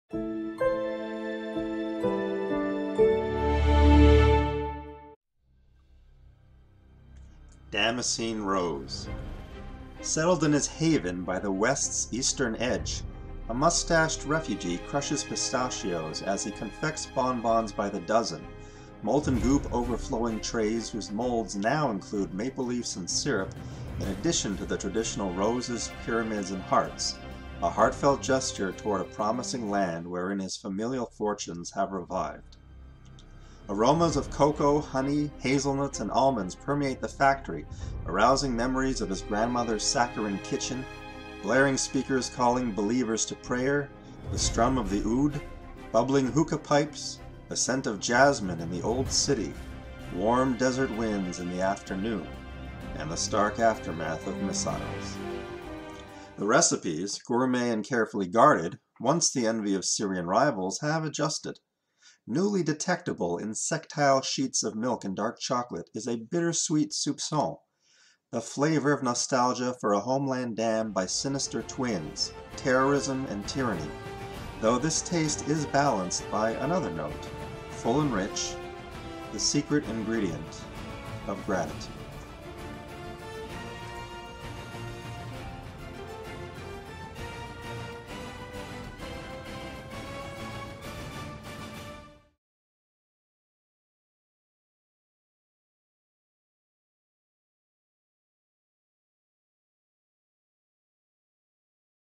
Damascene Rose by Brandon Marlon
"Damascene Rose"  was featured in our Unity issue (Vol 41, Issue 2).

Brandon Marlon is a writer from Ottawa, Canada. He received his B.A. in Drama & English from the University of Toronto and his M.A. in English from the University of Victoria. His poetry was awarded the Harry Hoyt Lacey Prize in Poetry (Fall 2015), and his writing has been published in 275+ publications in 30 countries.